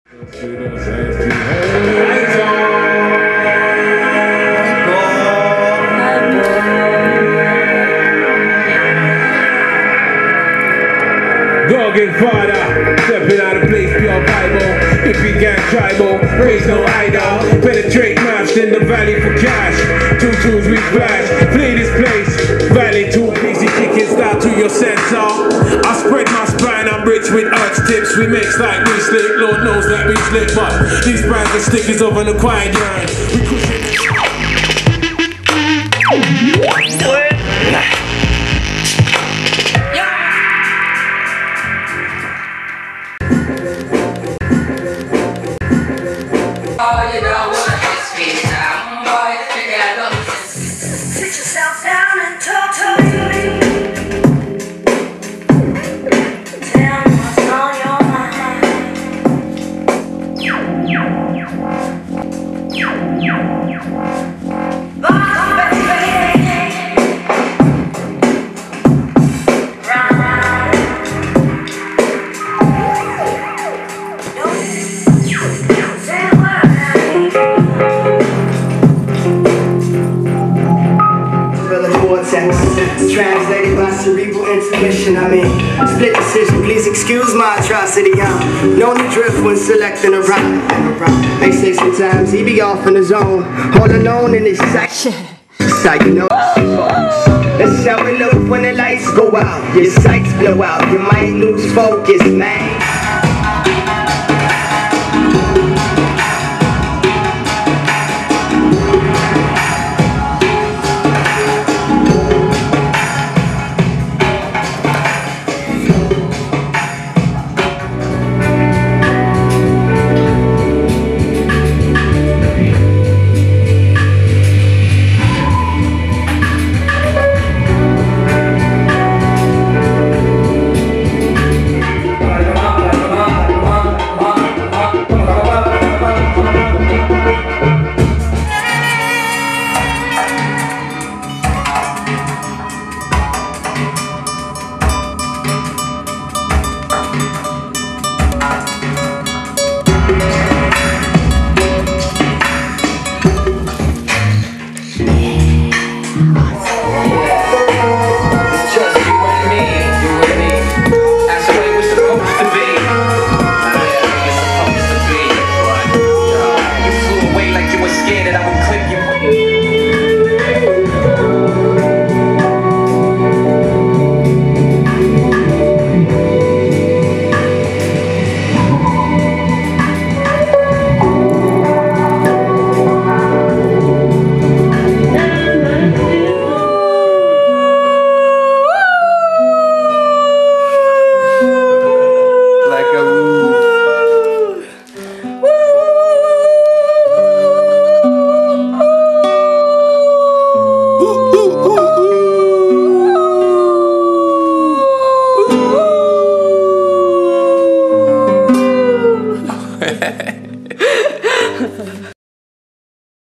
A wolf couchsurfing in Wien
This video was made during a couchsurf creative visit in Wien, 13 novembre 2009, with Fabian (dj set), Mathias (guitar), Francesca (dance).

Video shooting & editing: Francesca Ferrando